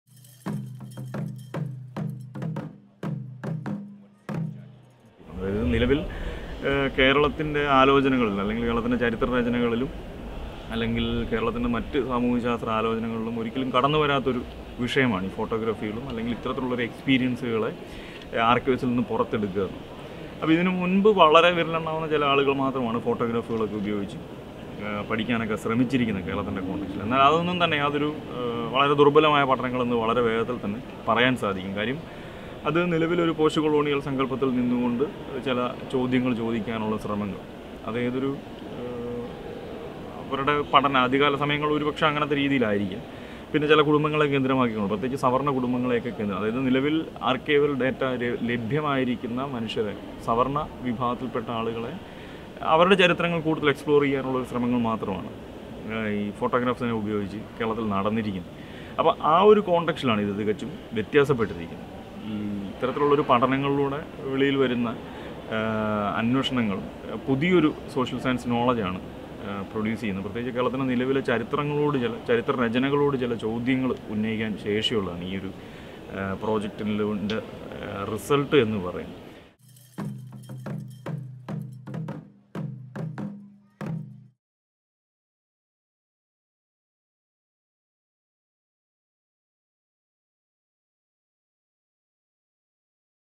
Wake up Calls for my Ancestors
Dr Vinil Paul Talks about Ethnography photography and its historical value on Kerala context.

OCTOBER 29, 12:00 - NOVEMBER 15, 19:00 Colonial archives in various museums and institutions across Europe generate diverse discourses related to history, memory, archiving, art and the authenticity of the colonized nations. „Wake up calls for my Ancestors“ is a long-term, critical artistic-archival project that gives an active voice to the silent voices of Dalit and other archived subaltern subjects, appropriated, exhibited, made accessible, edited, and disseminated as mere photographs . The artist Sajan Mani has initiated an interdisciplinary dialogue about the collection of South Indian photographs in the Ethnological Museum Berlin, to which he has invited three artists and three scholars to critically question Eurocentric archiving practices. Internal workshops, research work, installations, performances, films, photographs, panel discussions, interactions and experimental collaborations take place within the framework of the project in order to provide a conscious counterpoint to the accuracy of colonial photography. In search of a space and a history from below, „Wake up calls for my Ancestors“ challenges the widely accepted post-colonial methodology that deals with the great nation at the expense of smaller places like Keralam. The possibility of an everyday space-time that can connect both human and non-human actors for compassionate planetary justice orients this investigation to both futuristic and historical modes of thought.